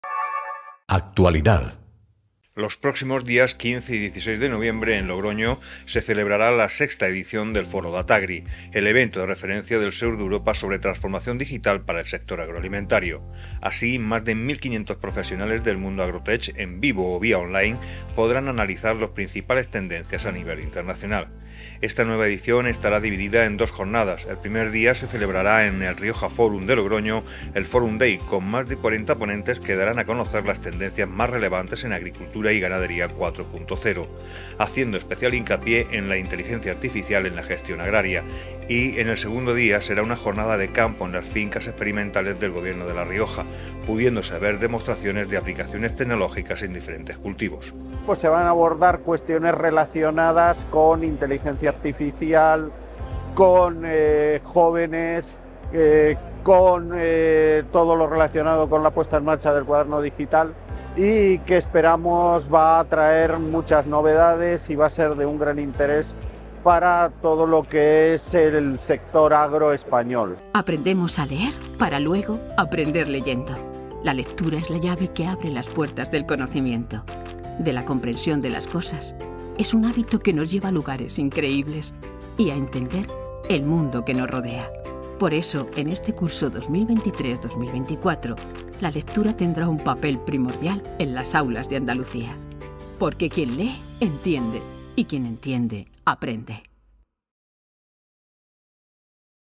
La innovación agraria llega a Logroño de la mano del Foro DATAGRI 2023
Los próximos días 15 y 16 de noviembre en Logroño se celebrará la VI edición del Foro DATAGRI, el evento de referencia del sur de Europa sobre transformación digital para el sector agroalimentario. Así, más de 1.500 profesionales del mundo agrotech, en vivo o vía online, podrán analizar las principales tendencias a nivel internacional. Esta nueva edición estará dividida en dos jornadas, el primer día se celebrará en el Rioja Fórum de Logroño el "Fórum Day" con más de 40 ponentes que darán a conocer las tendencias más relevantes en Agricultura y Ganadería 4.0, haciendo especial hincapié en la inteligencia artificial en la gestión agraria y, el segundo día será una jornada de campo en las fincas experimentales del Gobierno de la Rioja, pudiéndose ver demostraciones de aplicaciones tecnológicas en diferentes cultivos.